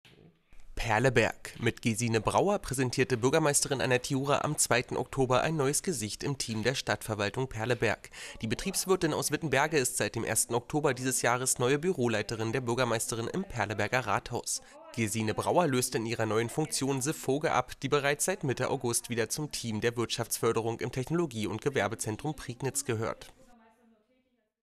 Perleberg: Gesine Brauer ist neue Büroleiterin von Annett Jura
Perleberg. Mit Gesine Brauer präsentierte Bürgermeisterin Annett Jura am 2. Oktober ein neues Gesicht im Team der Stadtverwaltung Perleberg. Die Betriebswirtin aus Wittenberge ist seit dem 1. Oktober dieses Jahres neue Büroleiterin der Bürgermeisterin im Perleberger Rathaus.